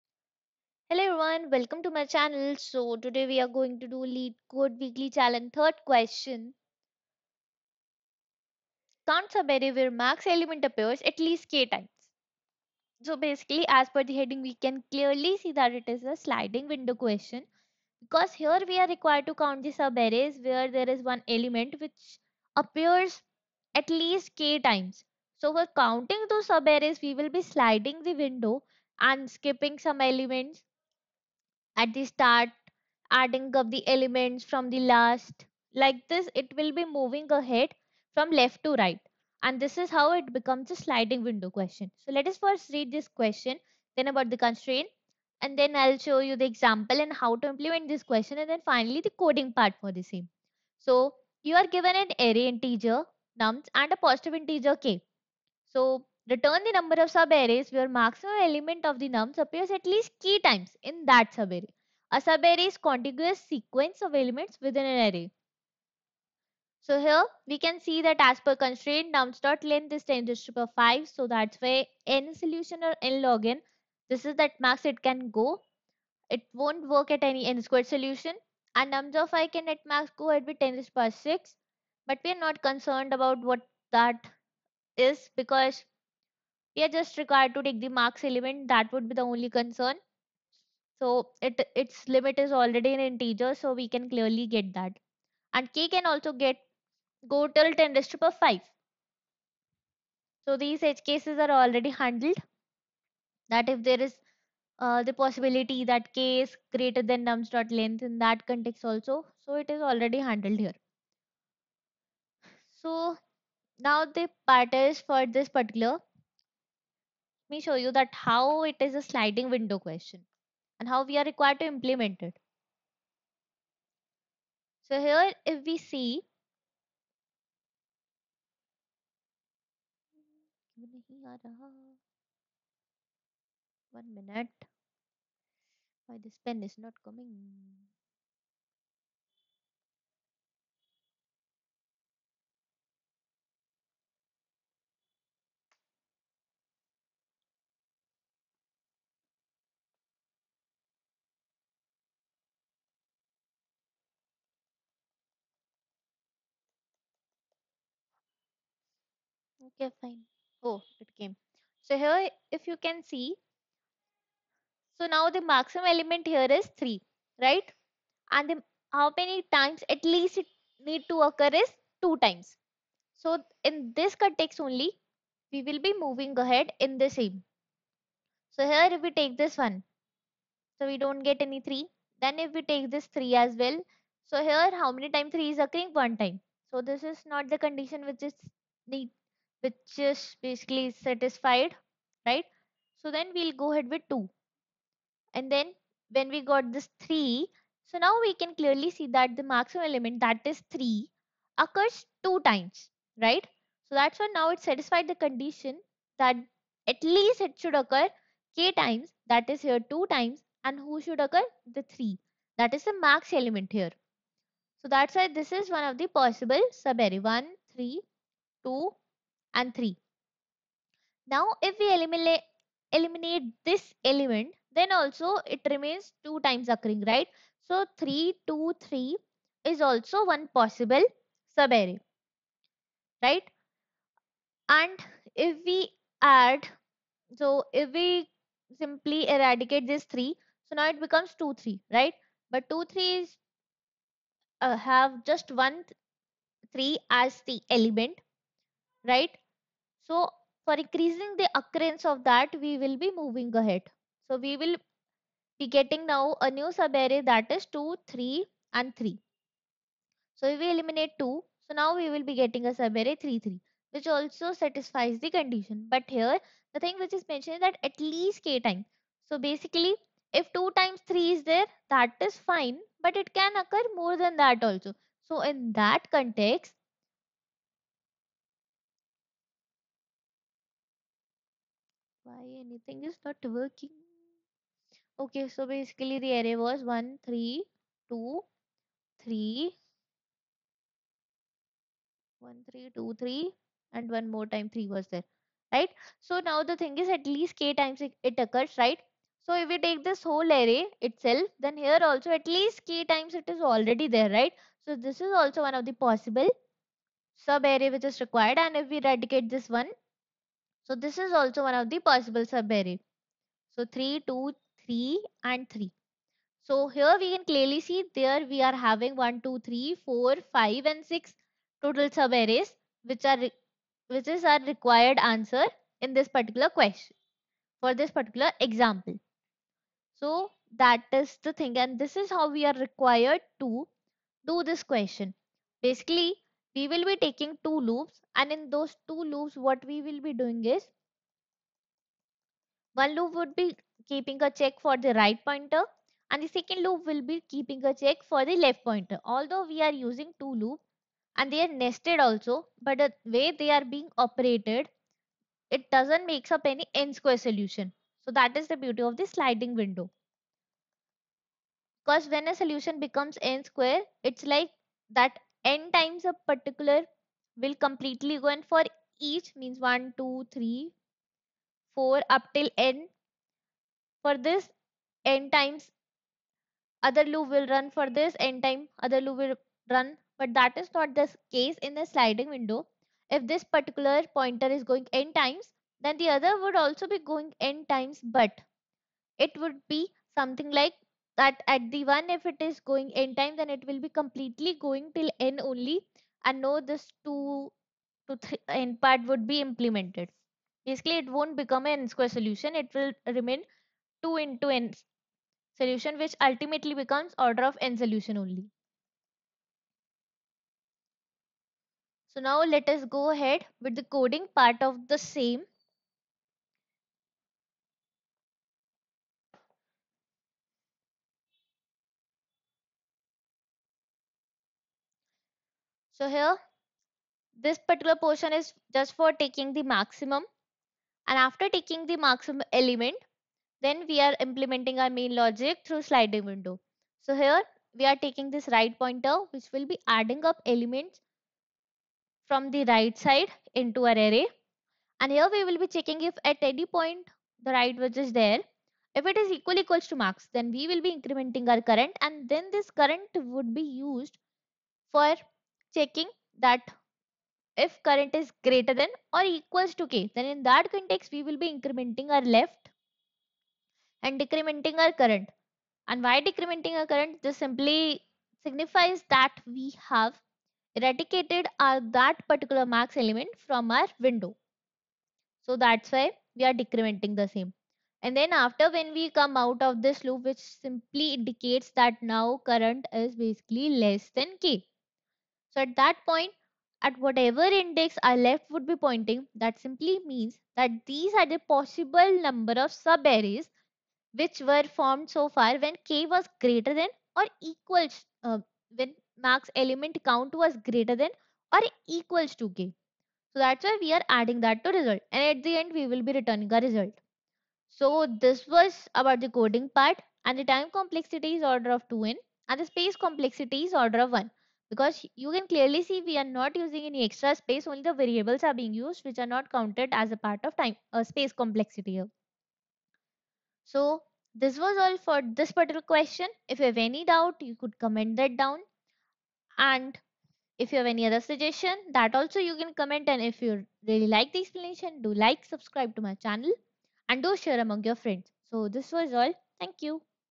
Leetcode 2962 || Count Subarrays Where Max Element Appears at Least K Times || Weekly Challenge
Tc: O(n)
SC: O(1)

If you have any doubt you can either comment or join my whatsapp community: